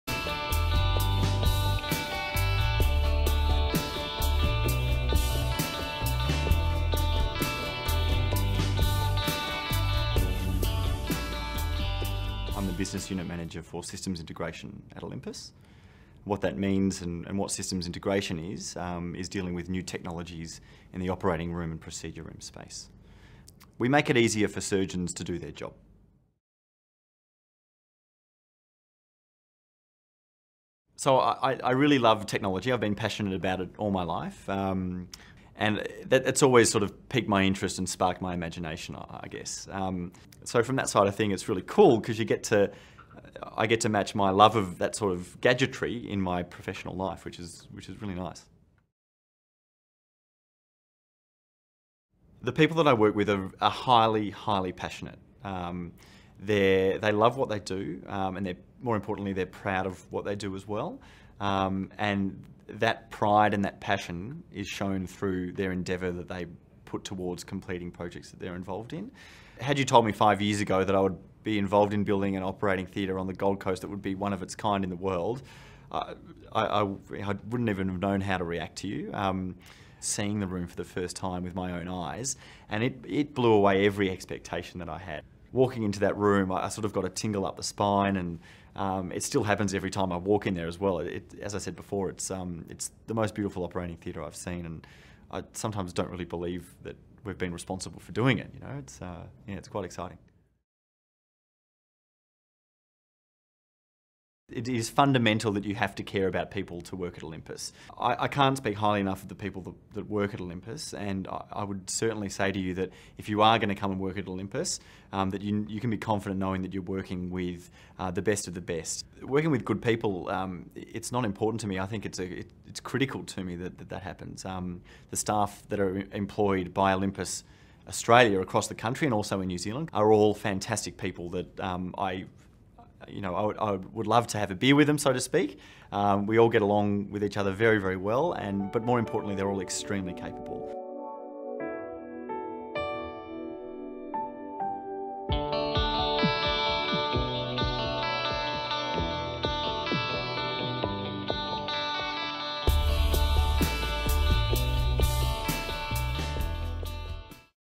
Working at Olympus - Oliver
Find out what it's like to work for Olympus Australia and New Zealand by watching this interview with one of our long tenured staff members Oliver Clarke.

At the time of the interview, Oliver was the Systems Integration Business Unit Manager, he is now the Communications Manager of Olympus Australia and New Zealand.